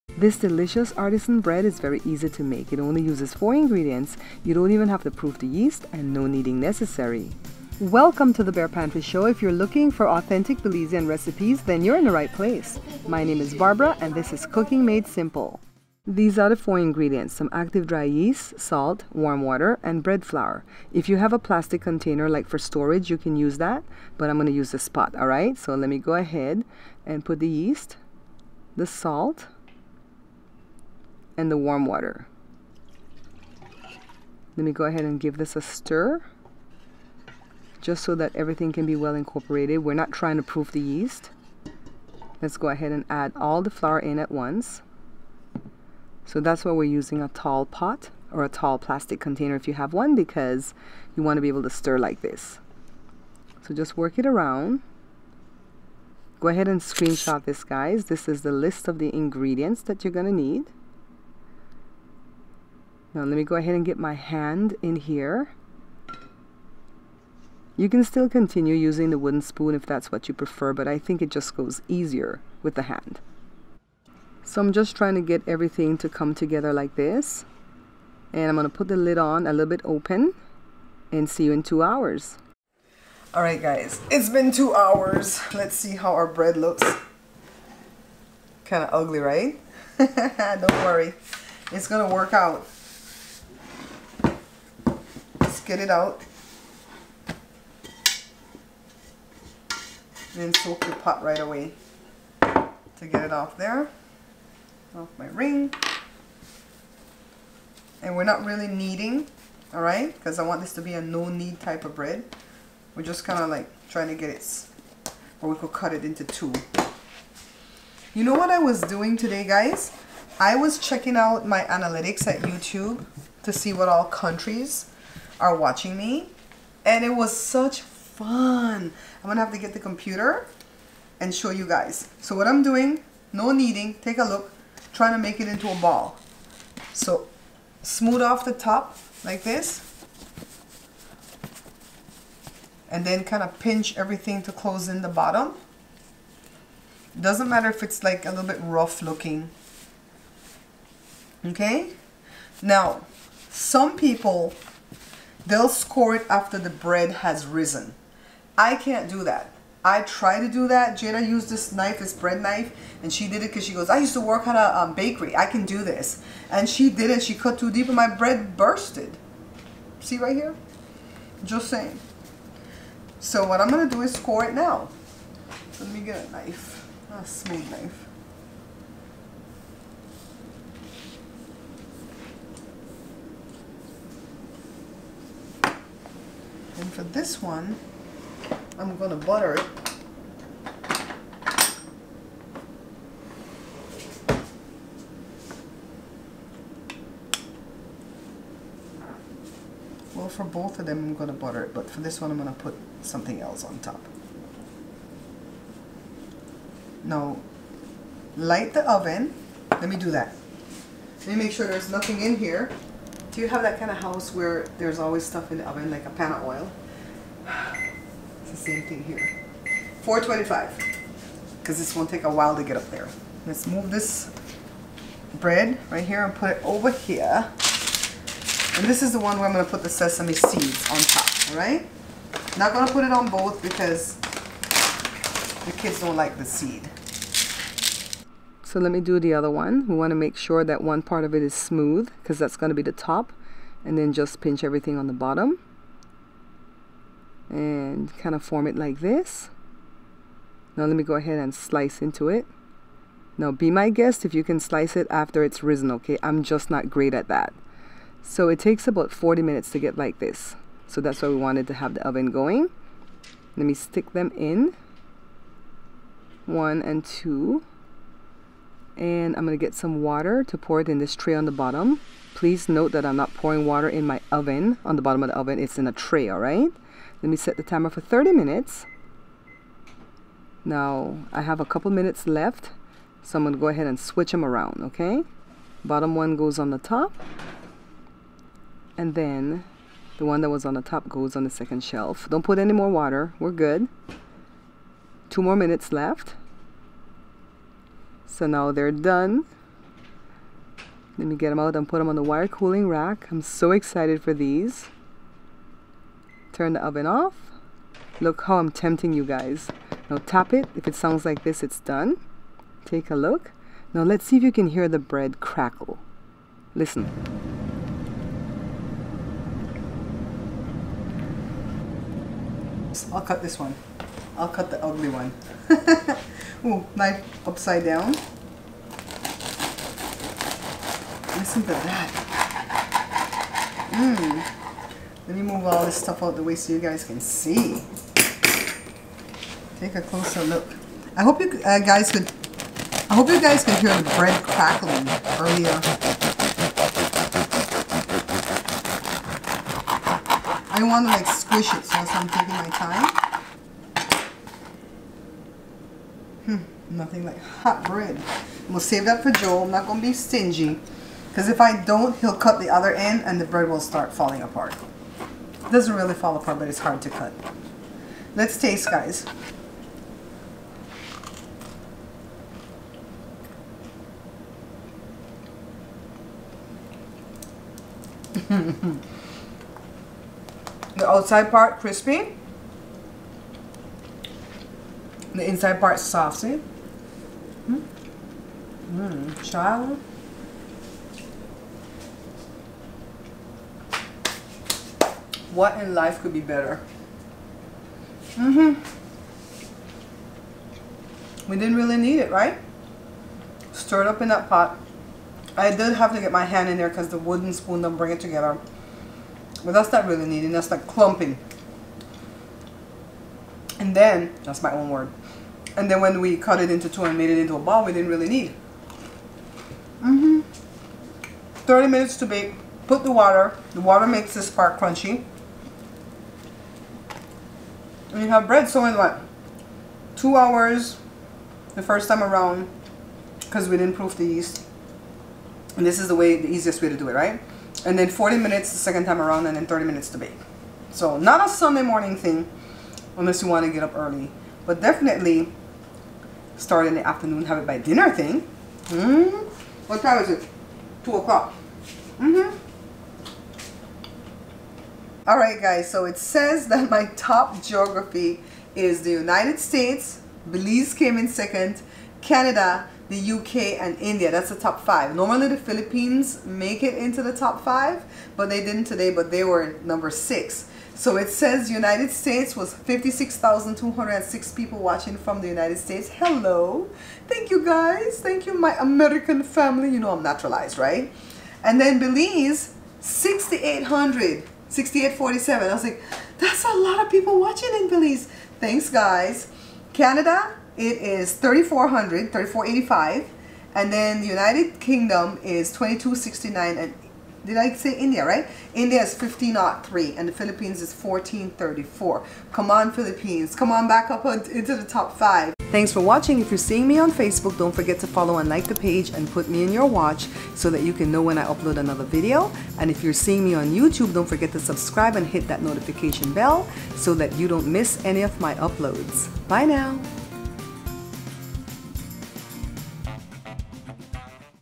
No Knead Artisan Bread
This Artisan bread uses only 4 ingredients and is very easy to make. No need to proof the yeast and no Kneading necessary.